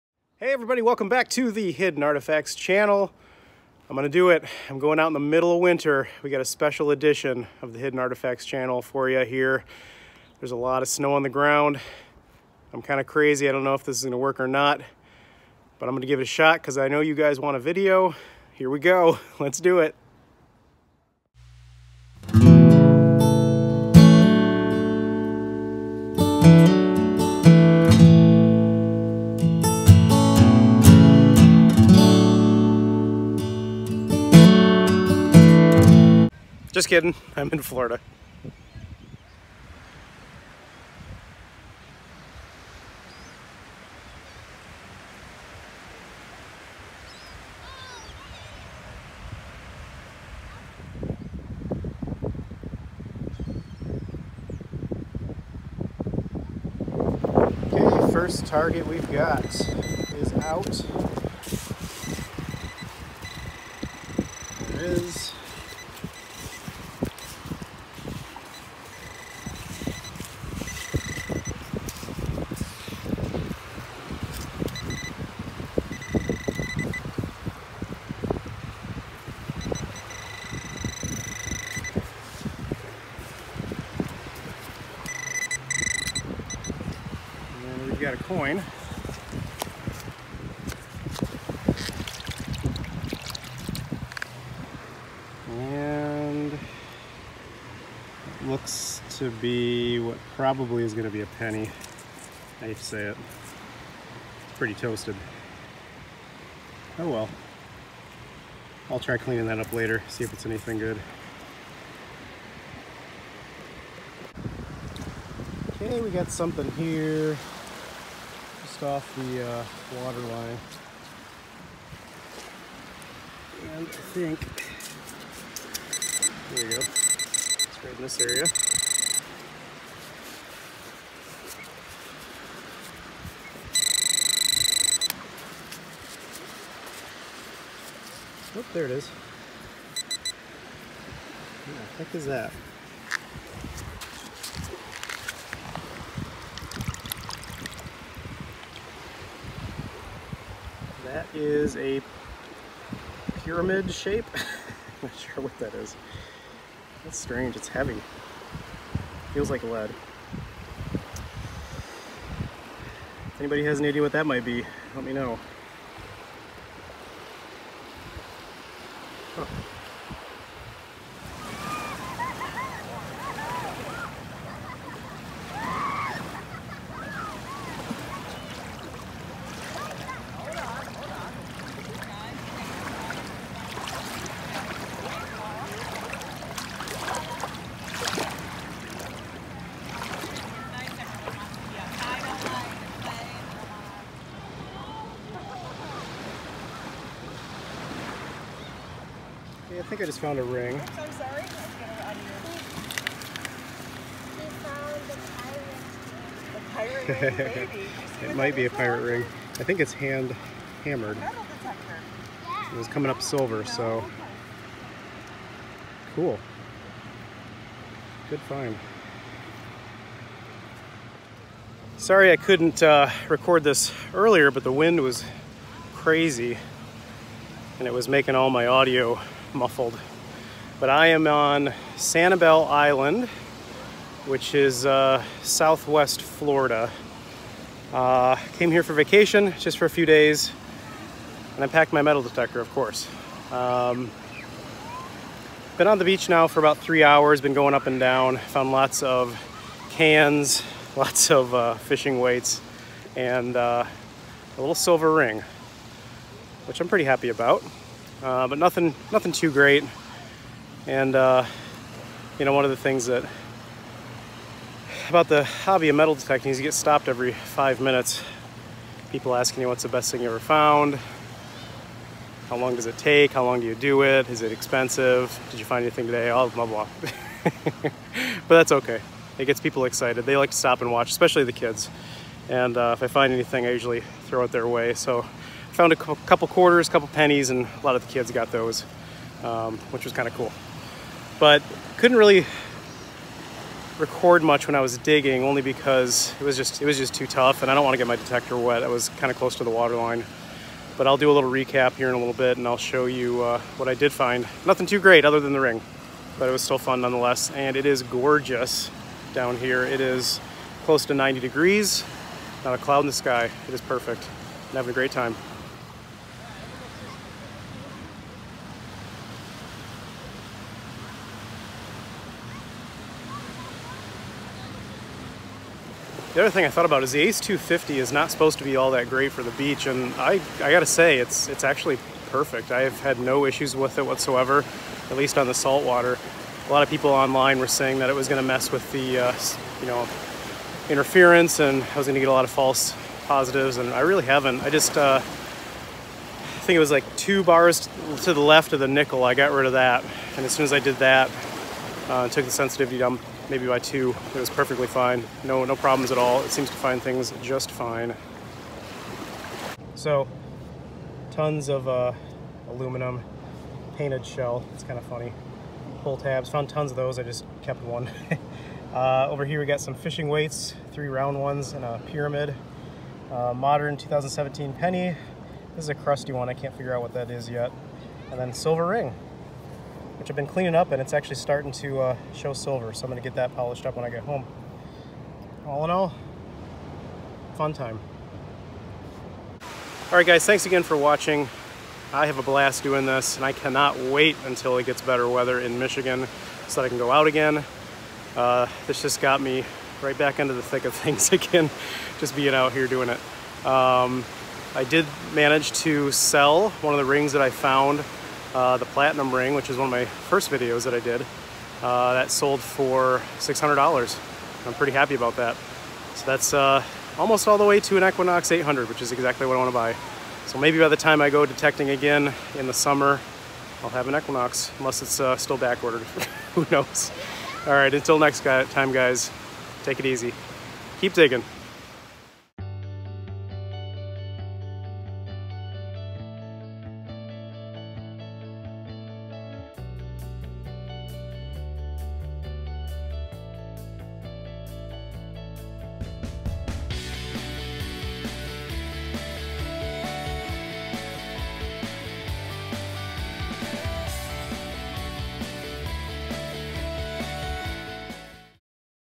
Hidden Artifacts - Winter Dig!
On this episode of The Hidden Artifacts Channel, I take you digging in the middle of Winter - which may not be what you are expecting...

Filmed in Michigan and Florida.

Camera used: Apple iPhone X
Detector Used: Garrett ACE250
Pinpointed Used: Deteknix

This video may not be copied or re-posted without my express written consent.